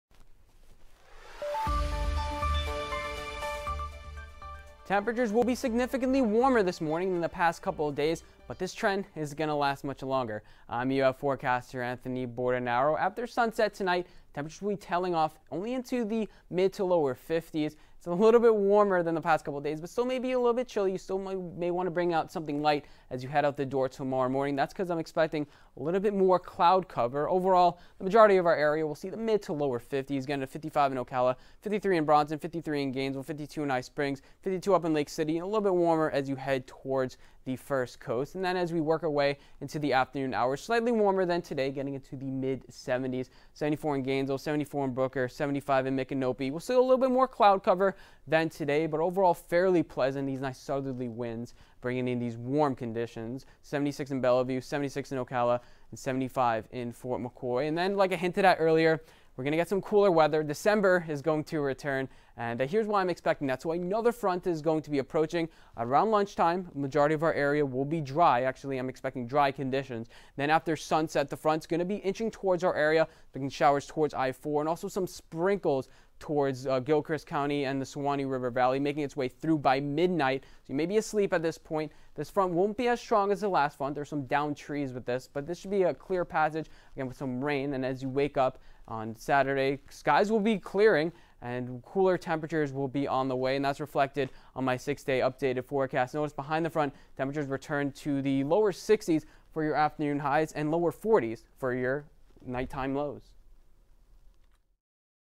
Local forecast for Gainesville-Ocala - December 3, 2020 4 pm ET
Local forecast as seen on WRUF-TV (Cox 1006 or 10.1) and published December 3, 2020 4 pm